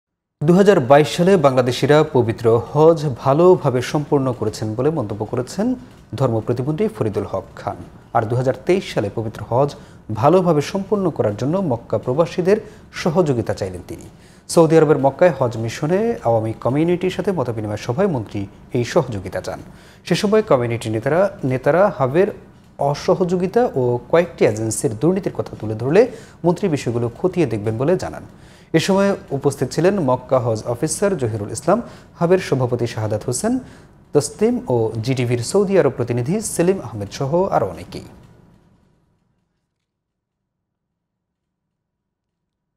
২০২৩ সালের হজে মক্কা প্রবাসীদের সহযোগিতা চাইলেন ধর্ম প্রতিমন্ত্রী | Faridul Haq Khan
২০২৩ সালের হজে মক্কা প্রবাসীদের সহযোগিতা চাইলেন ধর্ম প্রতিমন্ত্রী | Faridul Haq Khan